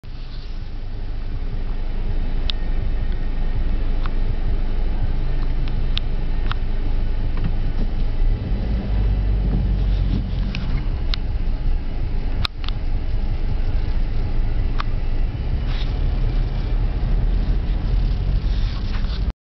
Caribou Reindeer -the northern cows
They stand around the roads like nothing up here. No care in the World.